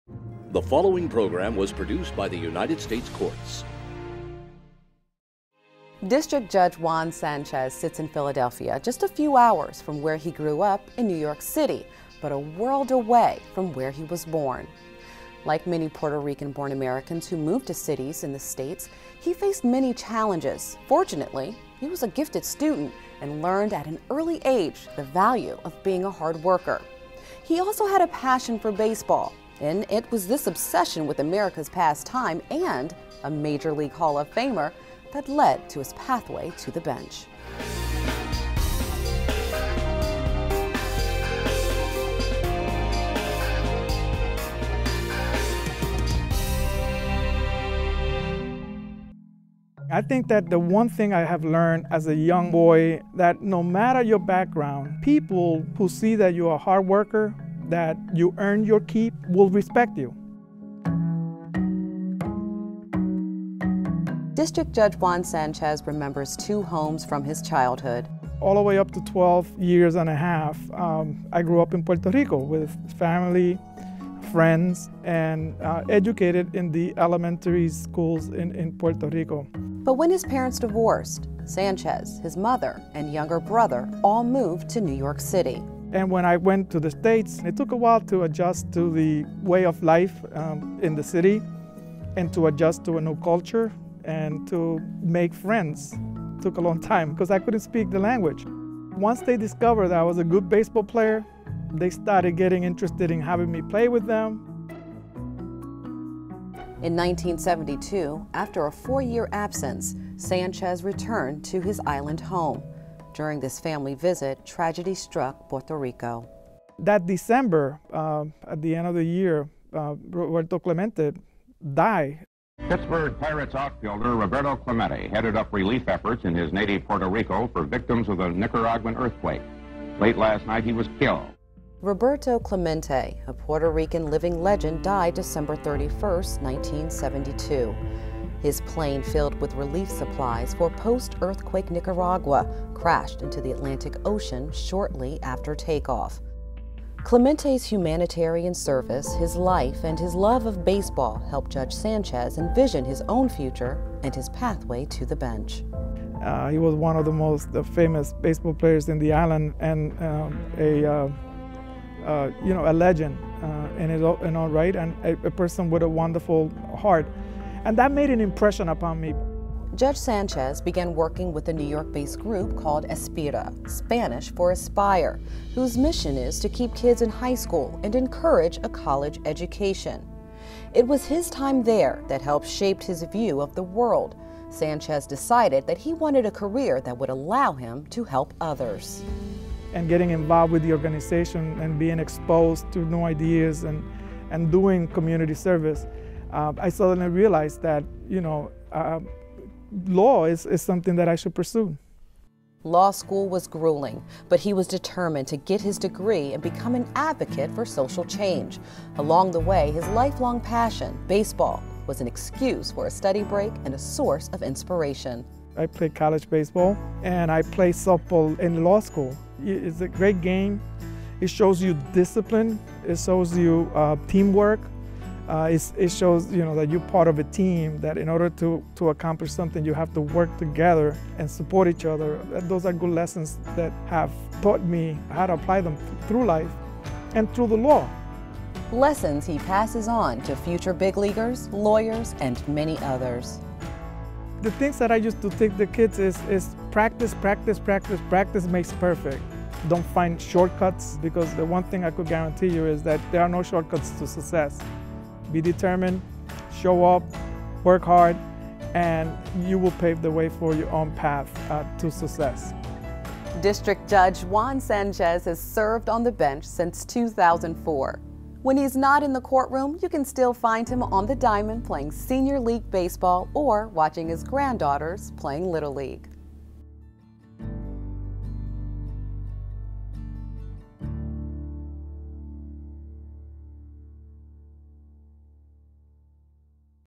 Pathways to the Bench: U.S. District Court Judge Juan Sánchez
U.S. District Court Judge Juan R. Sánchez, of Philadelphia, moved at age 12 from Puerto Rico to New York where he learned English, excelled in school, and developed his baseball talents. Baseball helped him assimilate and make friends, but it was his passion for public service that led him to the federal bench. He still plays baseball on a league and brings his work ethic and dedication to the diamond and to the courthouse every day.